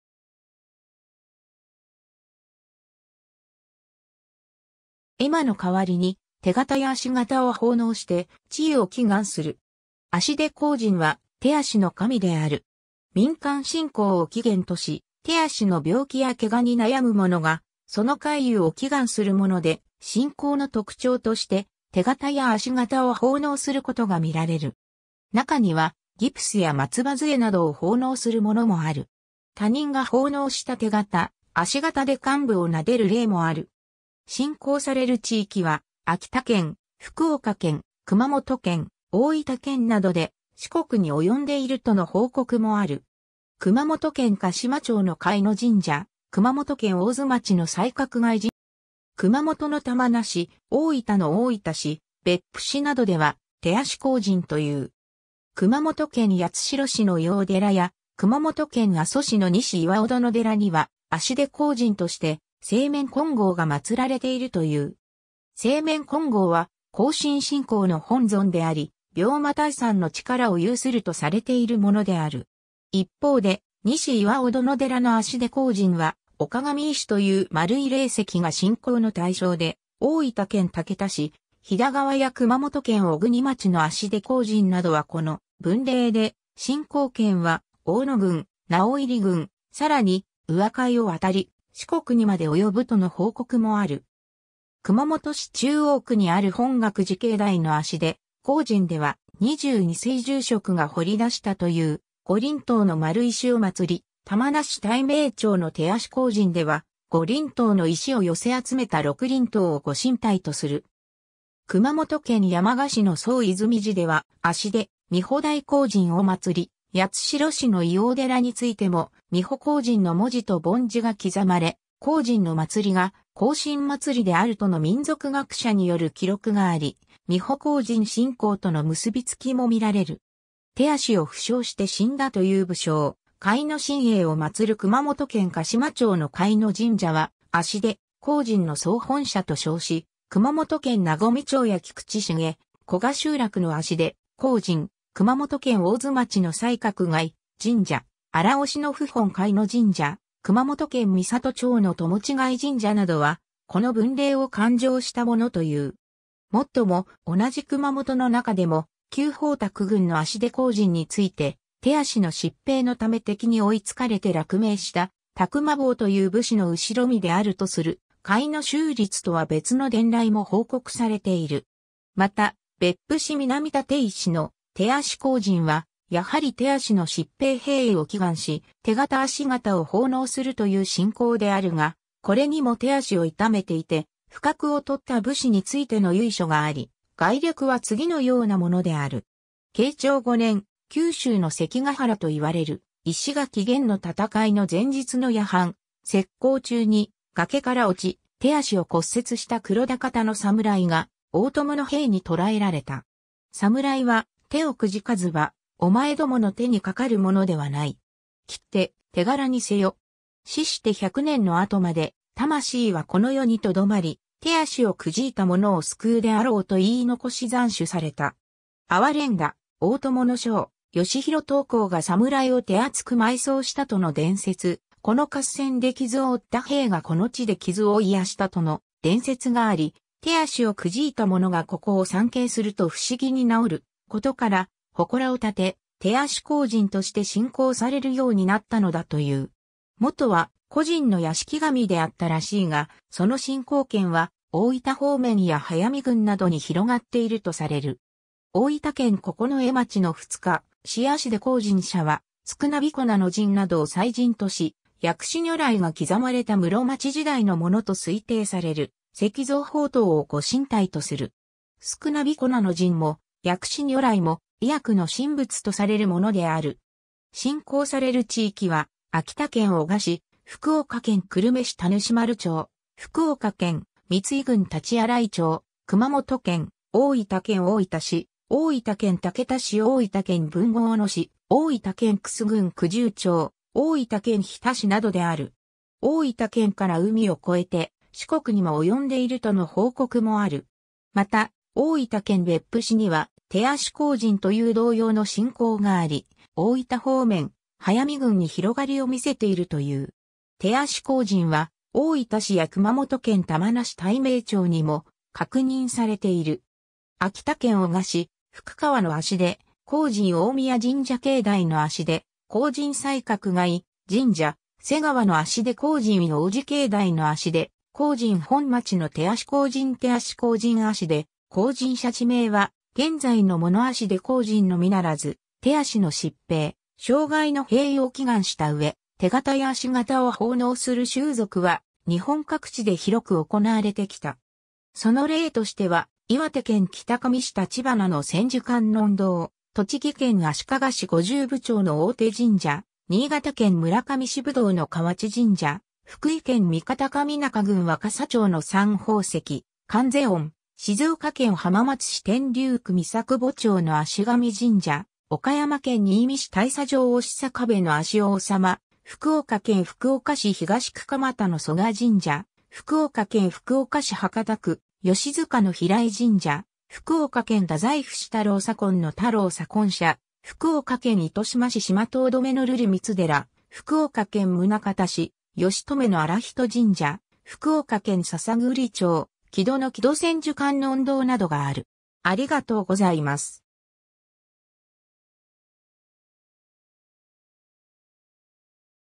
足手荒神
絵馬の代わりに手型や足型を奉納して治癒を祈願する。
（嘉島町の甲斐神社） 足手荒神（あしてこうじん）は、手足の神である。
民間信仰を起源とし、手足の病気や怪我に悩む者がその快癒を祈願するもので、信仰の特徴として手型や足型を奉納することが見られる。
中にはギプスや松葉杖などを奉納するものもある。
他人が奉納した手型、足型で患部をなでる例もある。
 信仰される地域は、秋田県、福岡県、熊本県、大分県などで、四国に及んでいるとの報告もある。
熊本県嘉島町の甲斐神社（中郡甲斐神社）、熊本県大津町の西鶴甲斐神社や、熊本県八代市の医王寺は、「足手荒神」と通称されている。
熊本の玉名市、大分の大分市、別府市などでは「手足荒神」と言う。
 熊本県八代市の医王寺や、熊本県阿蘇市の西巌殿寺には、足手荒神として青面金剛が祀られているという。
青面金剛は、庚申信仰の本尊であり、病魔退散の力を有するとされているものである。
 一方で、西巌殿寺の足手荒神はお鏡石という丸い霊石が信仰の対象で、大分県竹田市飛田川や熊本県小国町の足手荒神などはこの分霊で、信仰圏は大野郡、直入郡、さらに宇和海を渡り四国にまで及ぶとの報告もある。
 熊本市中央区にある本覚寺境内の足手荒神では、22世住職が掘り出したという五輪塔の丸石を祀り、玉名市岱明町の手足荒神では、五輪塔の石を寄せ集めた六輪塔をご神体とする。
 熊本県山鹿市の宗泉寺では、「足手三宝大荒神」を祀り、八代市の医王寺についても、三宝荒神の文字と梵字が刻まれ、荒神の祭りが庚申祭りであるとの民俗学者による記録があり、三宝荒神信仰との結びつきも見られる。
 手足を負傷して死んだという武将・甲斐親英（宗立）を祀る熊本県嘉島町の甲斐神社は足手荒神の総本社と称し、熊本県和水町や菊池市下古閑集落の足手荒神、熊本県大津町の西鶴甲斐神社、荒尾市の府本甲斐神社、熊本県美里町の砥用甲斐神社（現在廃社）などは、この分霊を勧請したものという。
 もっとも、同じ熊本の中でも、旧飽託郡の足手荒神について、手足の疾病のため敵に追いつかれて落命した託麻某という武士の後身であるとする、甲斐宗立とは別の伝来も報告されている。
 また、別府市南立石の「手足荒神」は、やはり手足の疾病平癒を祈願し、手型足型を奉納するという信仰であるが、これにも手足を痛めていて不覚をとった武士についての由緒があり、概略は次のようなものである。
慶長5年（1600年）、九州の関ヶ原と言われる石垣原の戦い（黒田如水 対 大友義統）の前日の夜半、斥候中に崖から落ち、手足を骨折した黒田方の侍が大友の兵に捕らえられた。
侍は、『手を挫かずば、お前共の手に掛かる者ではない。
切って手柄にせよ。
死して百年の後まで、魂はこの世にとどまり、手足を挫いた者を救うであろう』と言い残し斬首された。
哀れんだ大友の将・吉弘統幸が侍を手厚く埋葬したとの伝説、この合戦で傷を負った兵がこの地で傷を癒したとの伝説があり、手足を挫いた者がここを参詣すると不思議に治ることから、祠を建て「手足荒神」として信仰されるようになったのだという。
元は個人の屋敷神であったらしいが、その信仰圏は大分方面や速見郡などに広がっているとされる。
 大分県九重町の二日市足手荒神社は、少彦名神などを祭神とし、薬師如来が刻まれた室町時代のものと推定される石造宝塔をご神体とする。
少彦名神も薬師如来も、医薬の神仏とされるものである。
 信仰される地域は、秋田県男鹿市、福岡県久留米市田主丸町、福岡県三井郡大刀洗町、熊本県、大分県大分市、大分県竹田市大分県豊後大野市、大分県玖珠郡九重町、大分県日田市などである。
大分県から海を越えて四国にも及んでいるとの報告もある。
また、大分県別府市には、「手足荒神」という同様の信仰があり、大分方面、速見郡に広がりを見せているという。
「手足荒神」は大分市や熊本県玉名市岱明町にも確認されている。
 秋田県男鹿市福川の足手荒神 大宮神社境内の足手荒神（熊本県山鹿市） 西鶴甲斐神社（熊本県菊池郡大津町） 瀬川の足手荒神（熊本県水俣市） 医王寺境内の足手荒神（熊本県八代市） 本町の手足荒神（大分県大分市大字野津原） 手足荒神（大分県別府市南立石） 足手荒神社（大分県竹田市飛田川字天神） ※ 地名は現在のもの 足手荒神のみならず、手足の疾病・傷害の平癒を祈願した上、手型や足型を奉納する習俗は、日本各地で広く行われてきた。
その例としては、 岩手県北上市立花の千手観音堂、 栃木県足利市五十部町の大手神社、 新潟県村上...